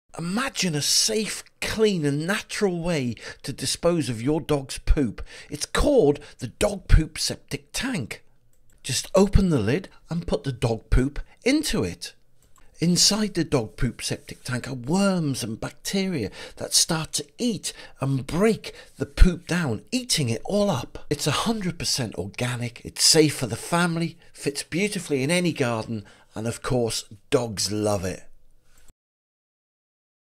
Dog Poop Worms
Do you want a safe easy way to dispose of your dogs poop?
Then the dog poop septic tank is for you.